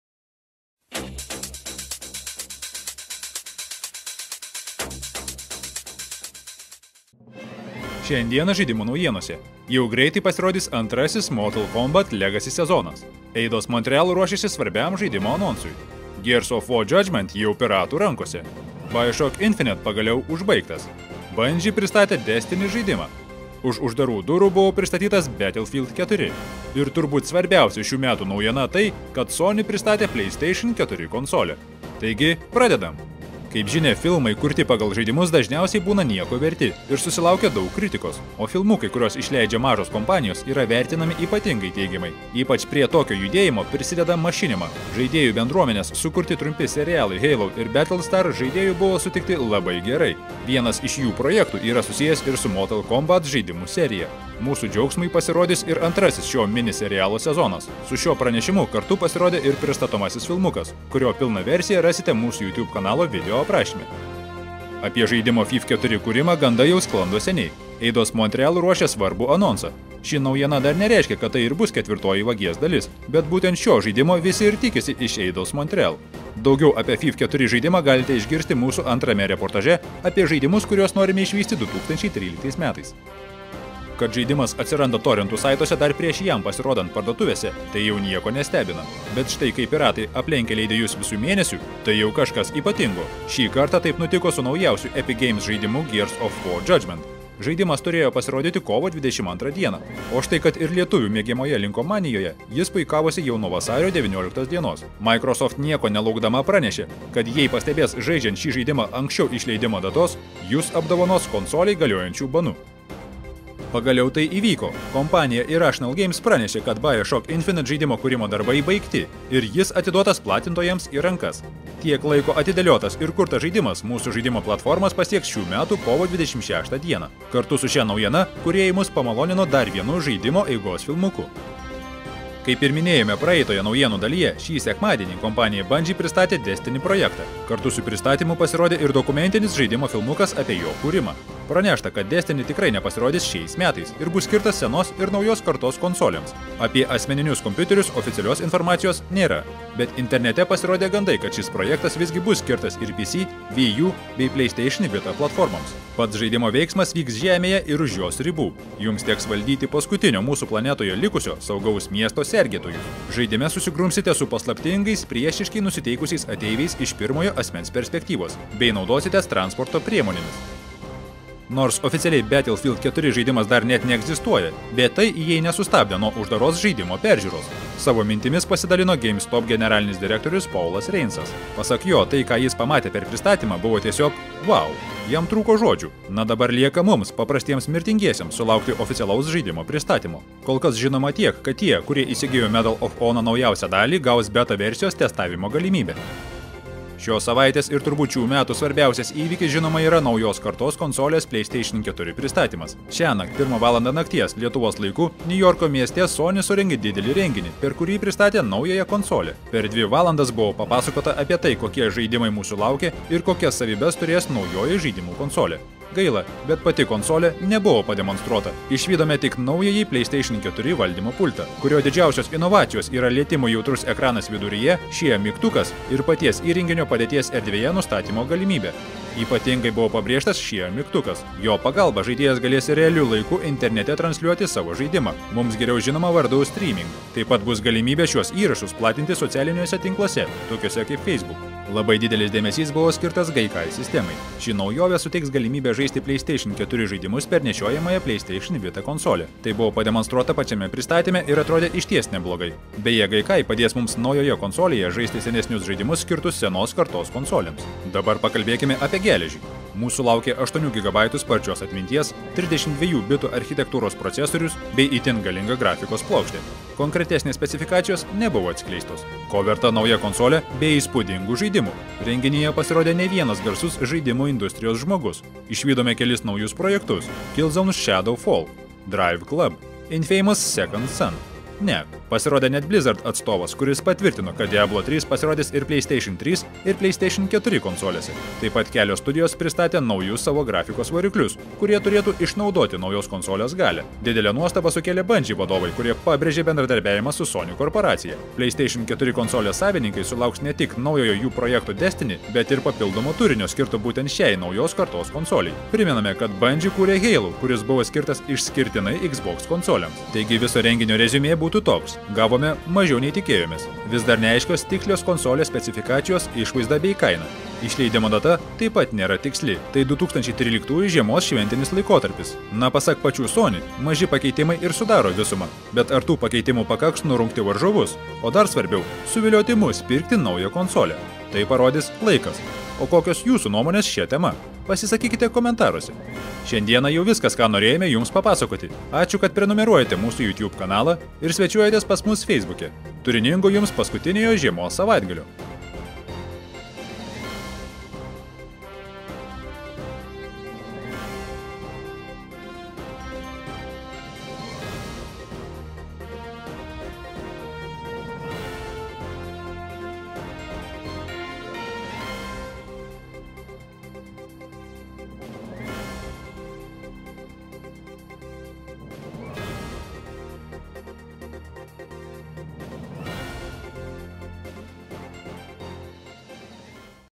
Žaidimų naujienos 2013.02.21
Žaidimų Balsas ieško Web dizainerio, logotipų ir video intro kūrėjų.

Patinka Žaidimų Balso darbai? Tapk mūsų draugu
Paremkite mus ir padėkite toliau plėtoti projektą AČIŪ

Šiandieną žaidimų naujienose:
Jau greitai pasirodys antrasis Mortal Kombat: Legacy sezonas;
Eidos Montreal ruošiasi svarbiam žaidimo anonsui;
Gears of War: Judgment jau piratų rankose;
BioShock Infinite pagaliau užbaigtas;
Bungie pristatė Destiny žaidimą;
Už uždarų durų buvo pristatytas Battlefield 4;
Ir turbūt svarbiausia šių metų naujiena tai, kad SONY pristatė PlayStation 4 konsolę.